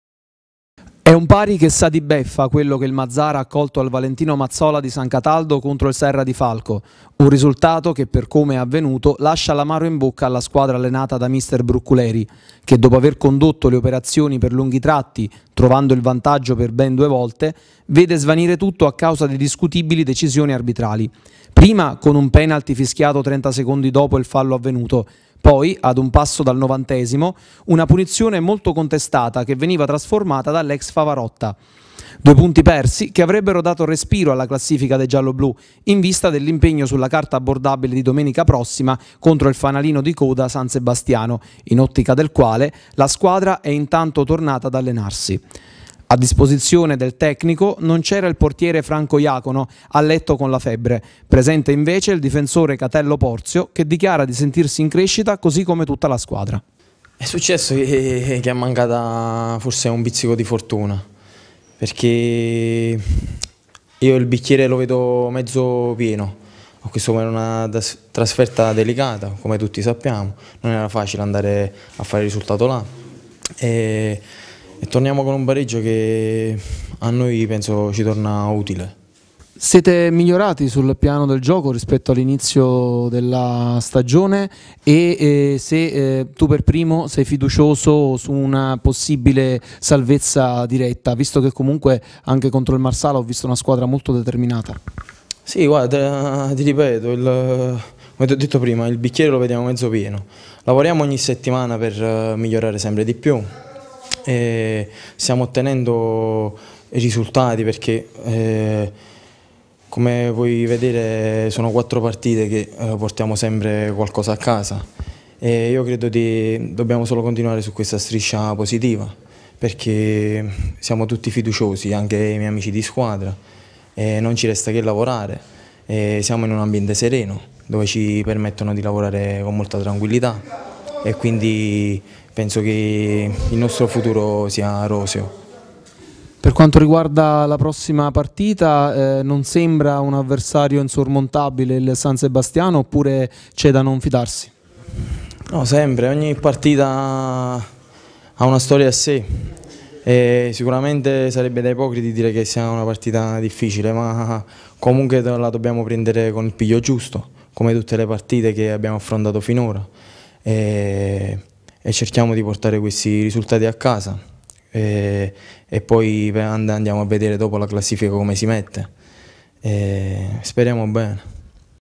2014 03 09 Per il Mazara calcio la beffa dell'ex
Per il Mazara calcio la beffa dell'ex. Servizio di Tommaso Ardagna